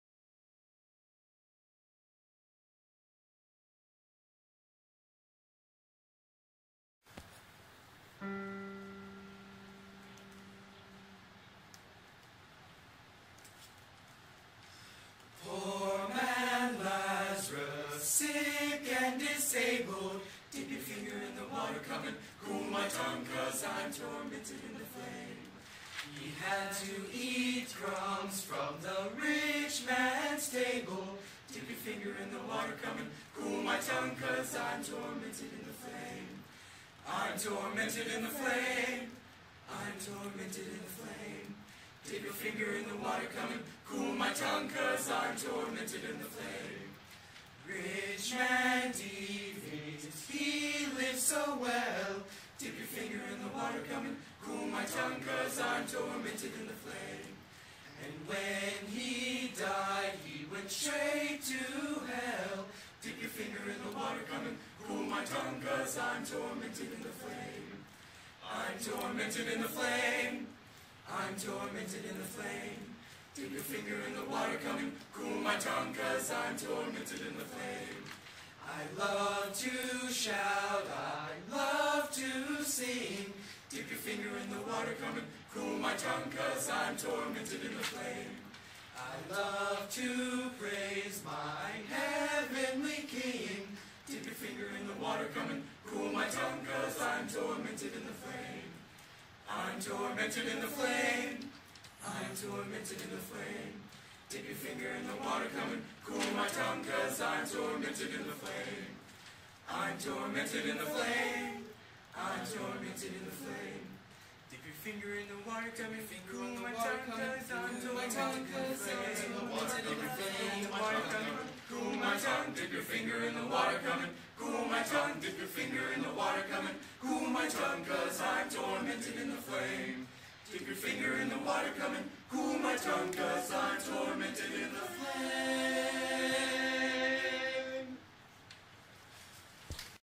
Lewis Cass Men's Ensemble ISSMA 2014
The Lewis Cass Men's Ensemble from Lewis Cass Jr/Sr High, at the regional ISSMA 2014 competition. 2/1/2014